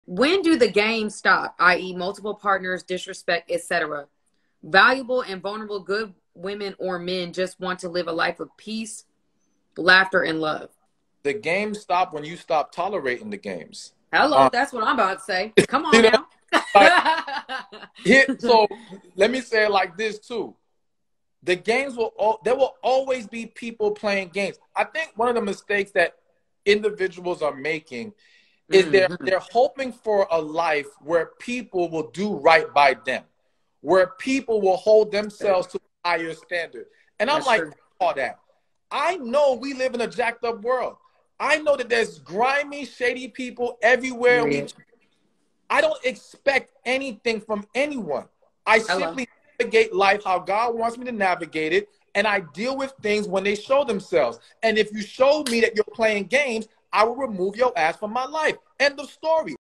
When Do People Stop Playing Games In Dating & Relationships?
❤️ Here’s your chance to get your personal questions answered for FREE

When do people stop playing games in dating and relationships?